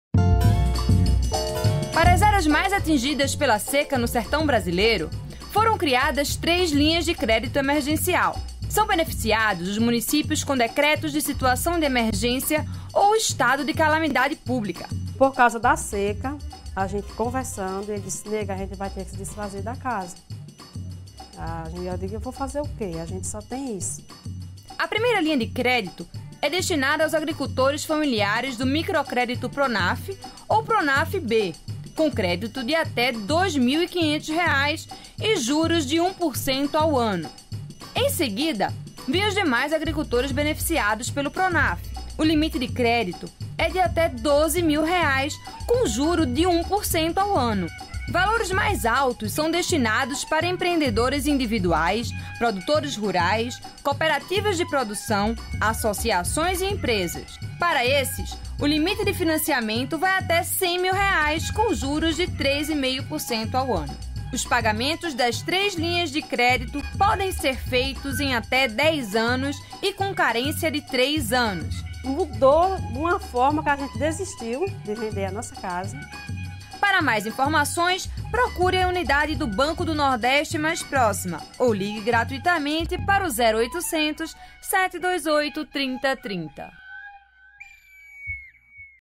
Agricultores de municípios afetados pela seca podem obter linha de crédito emergencial
Para as áreas mais atingidas pela seca no sertão brasileiro, foram criadas três linhas de crédito emergencial. São beneficiados os municípios com decretos de situação de emergência ou estado de calamidade pública. O acesso ao crédito contempla desde agricultores familiares até empresas. Veja no vídeo como se beneficiar.